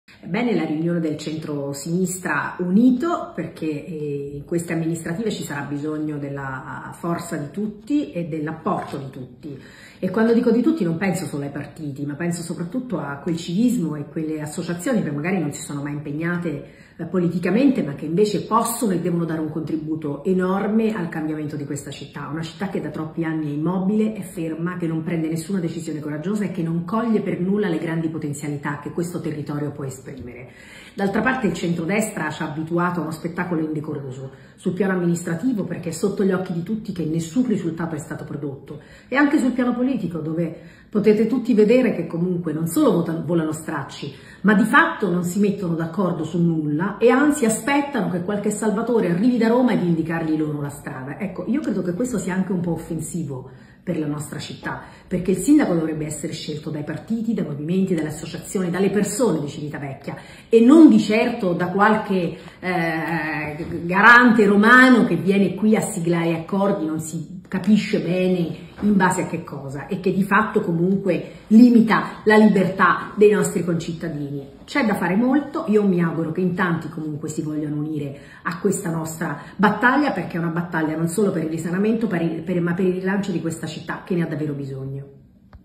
Tidei (Iv): "Costruire una proposta unitaria alternativa a una giunta di centrodestra fallimentare”
“A Civitavecchia stiamo lavorando per costruire una proposta unitaria e in assoluta discontinuità con l’amministrazione Tedesco, che in cinque anni non ha prodotto alcun risultato tangibile in nessun settore”. Lo afferma in una nota la consigliera regionale Marietta Tidei.